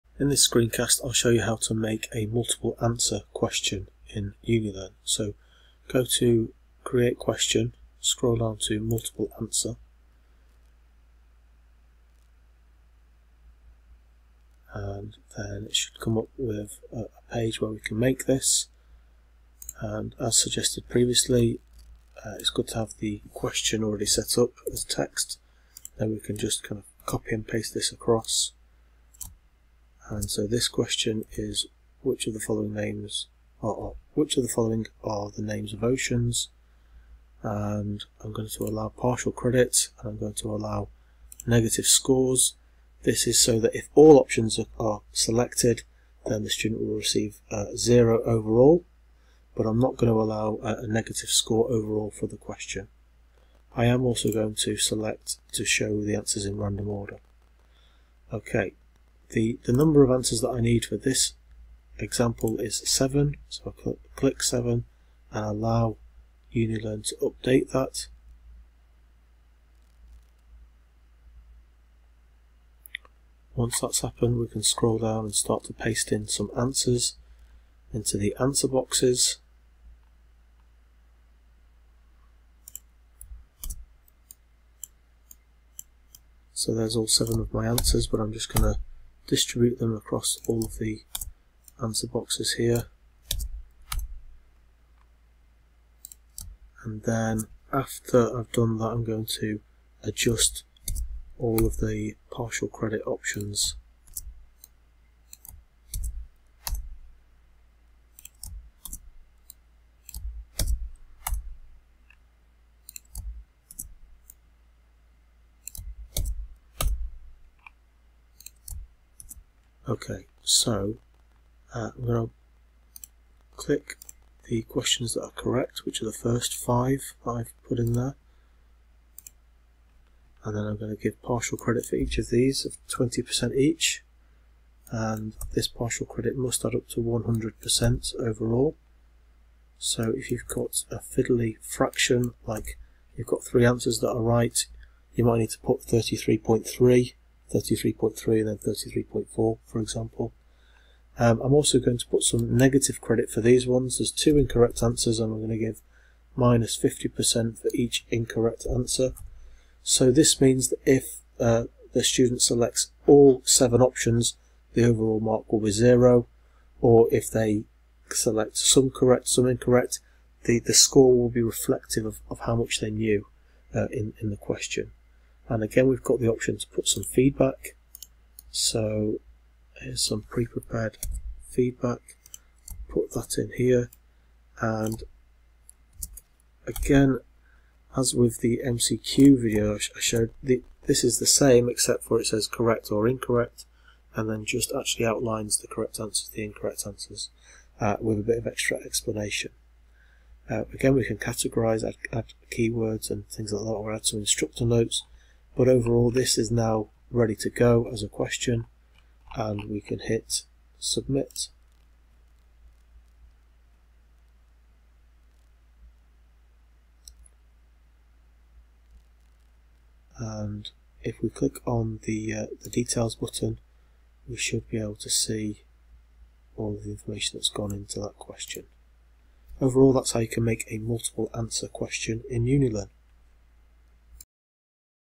How to create MAQs in UniLearn (Blackboard Learn)
How to create multiple answer questions (MAQs) in UniLearn (based on Blackboard Learn VLE).
Presented by Dr Daniel Belton, Senior Lecturer, University of Huddersfield